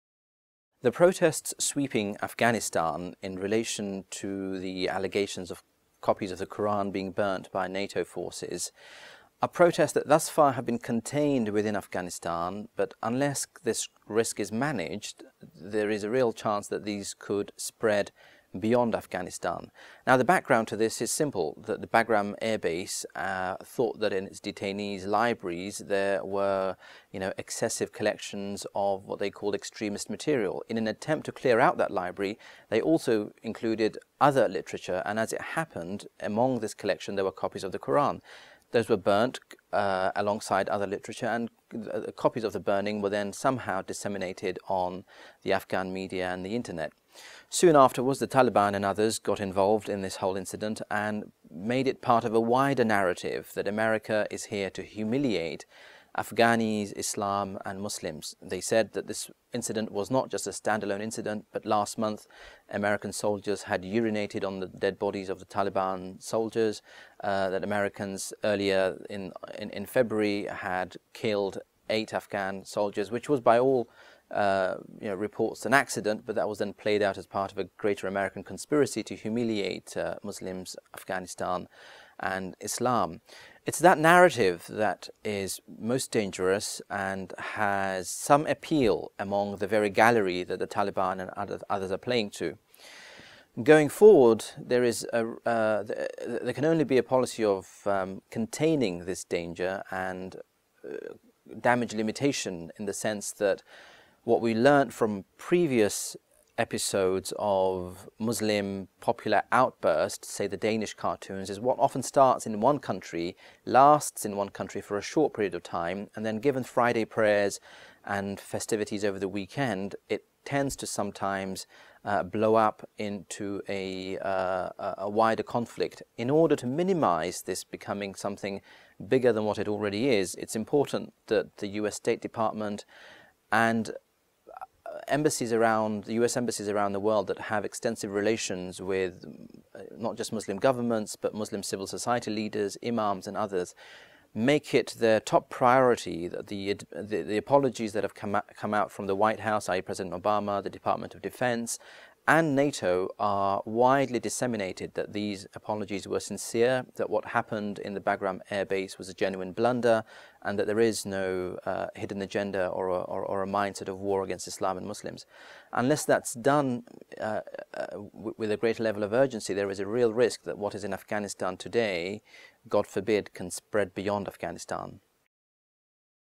Protests Over Quran Burning May Spread Beyond Afghanistan
The protests sweeping Afghanistan following the burning of Qurans by NATO forces could spread beyond Afghanistan into other Muslim and Arab countries, says CFR's Senior Fellow for Middle Eastern Studies Ed Husain.

"What often starts in one country lasts in one country for a short period of time," says Husain, and may then "blow up into a wider conflict." This was the case in previous episodes of Muslim popular outbursts, such as the 2005 controversy over a Danish cartoon depicting the Prophet Muhammad, he recalls.

"The Taliban and others got involved in this whole incident and made it part of a wider narrative that America is here to humiliate Afghanis, Islam, and Muslims," Husain explains. The danger is that this narrative appeals to the very audience that the Taliban and others are targeting, he says.

In order to minimize the damage and mitigate the spread of protests, Husain says U.S. and NATO officials must reach out not only to Muslim governments but also to Muslim civil society and religious leaders. The official White House, Department of Defense, and NATO apologies must be widely disseminated, and it must be made clear that "there is no hidden agenda or a mindset of war against Islam and Muslims," he adds.

For more analysis from Ed Husain, read his blog The Arab Street